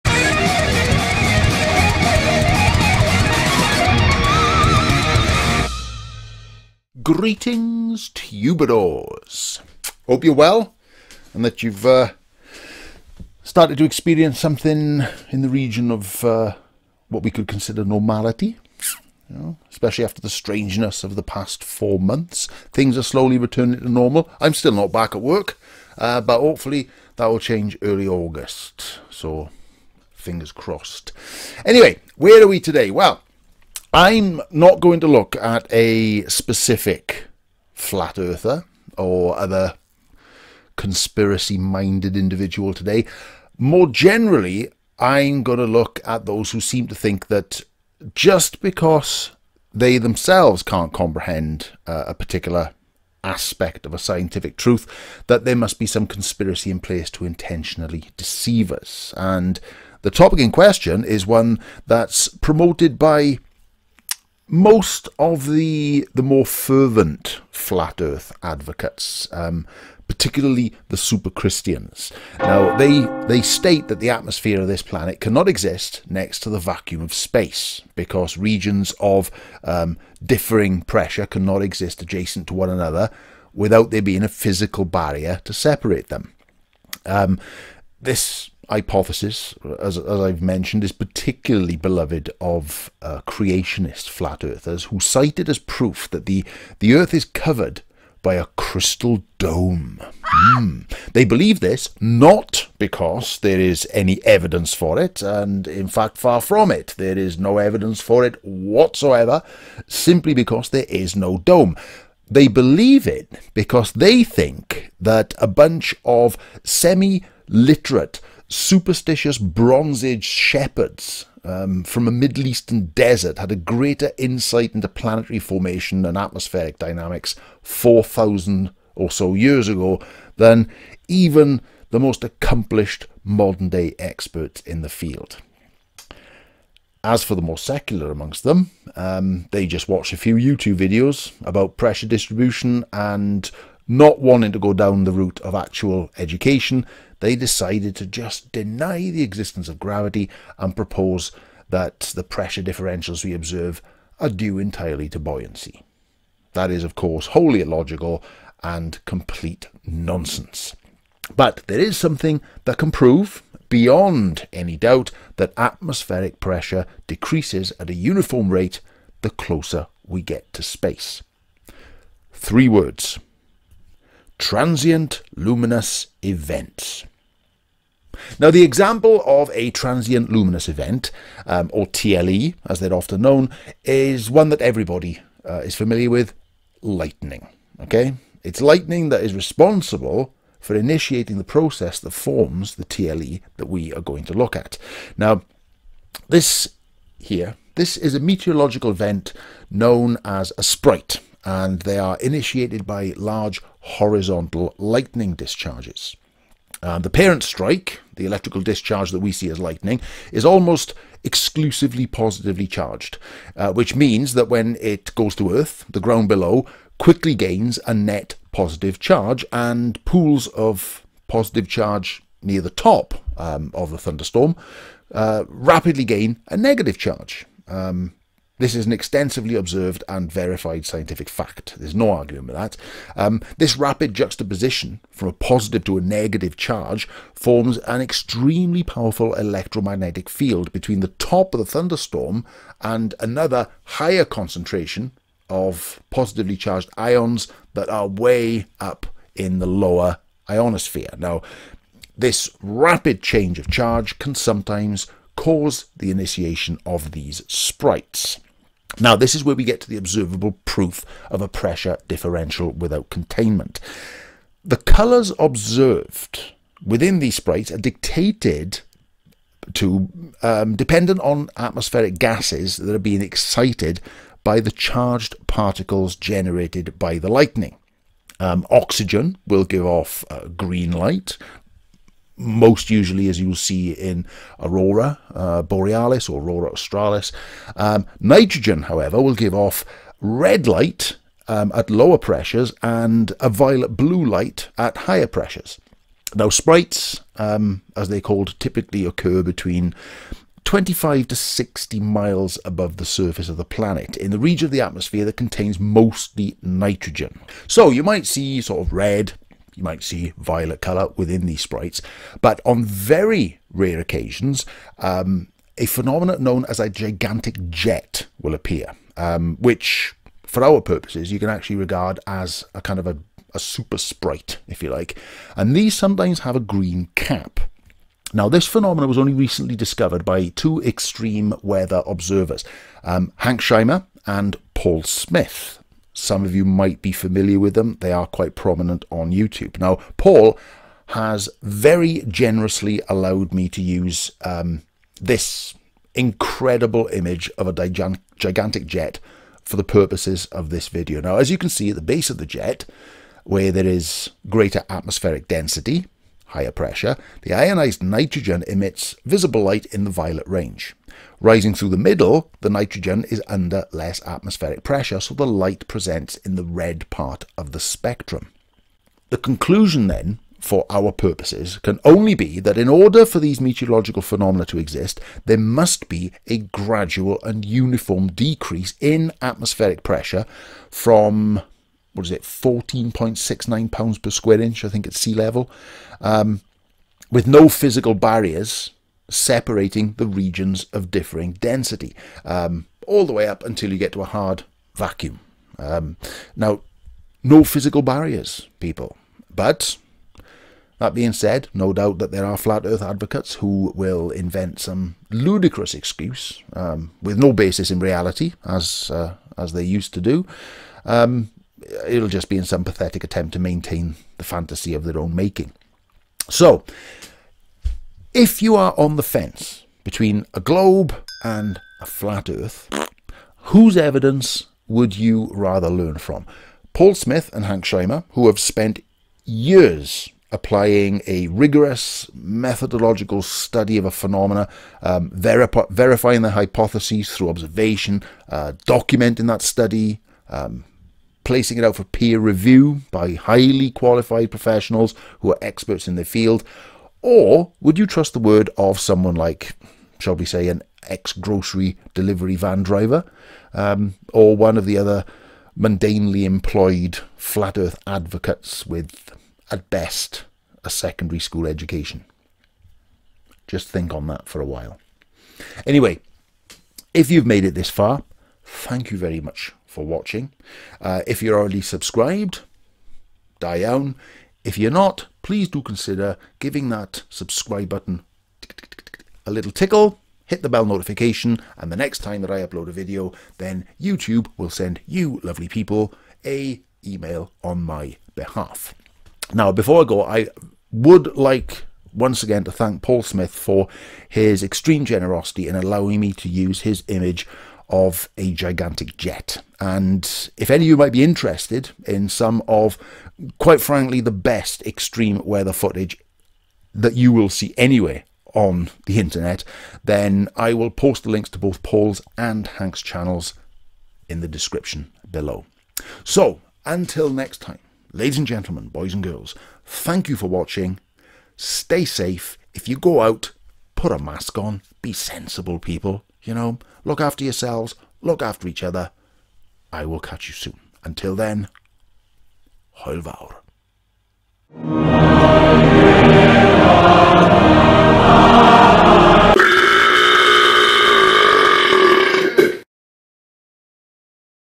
FLAT EARTH BELIEVERS ARE GOING TO HATE THIS.
One of the most ludicrous claims from Flat Earthers, Ultra-Fundamentalist Creationists, conspiracists, and general idiots, is that Earths atmosphere cannot exist next to the vacuum of space.
It can and it does. Here is visual proof that our atmosphere varies in pressure from 14.69 lbs p.s.i at sea level, to the near-vacuum of space.

Special thanks to extreme weather observer Paul Smith, who gave special permission to use one of his amazing photographs for use in this video.